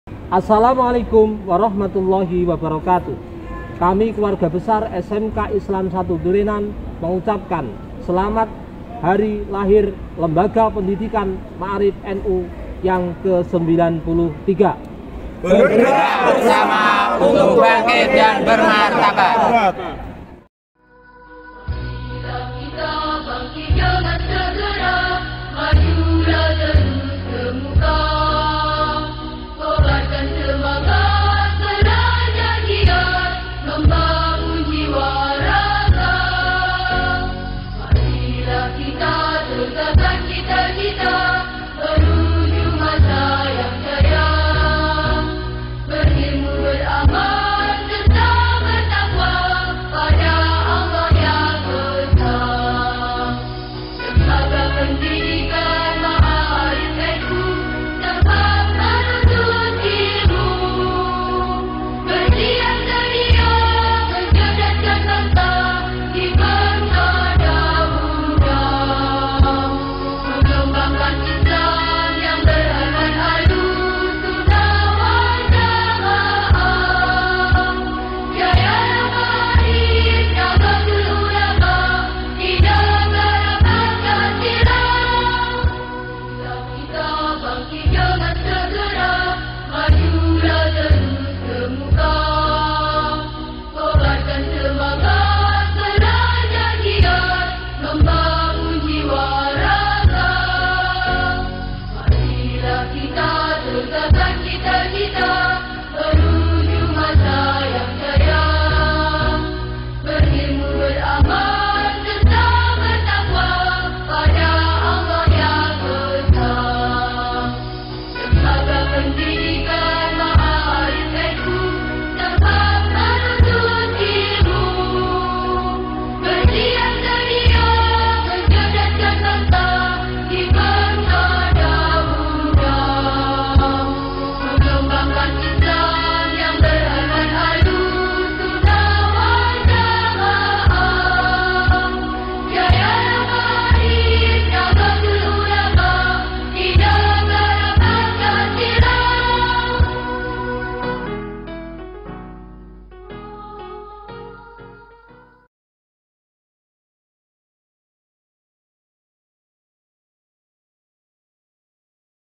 UPACARA MEMPERINGATI HARI LAHIR LP MA'ARIF NU Ke - 93 (SENIN 19 SEPTEMBER 2022) SMK ISLAM 1 DURENAN
Upacara memperingati Hari Lahir Lembaga Pedidikan (LP) Ma'arif NU Ke - 93 Tahun 2022 di SMK Islam 1 Durenan dengan Pembina Bapak Drs. H. Mukholis, MM selaku Kepala SMK Islam 1 Durenan.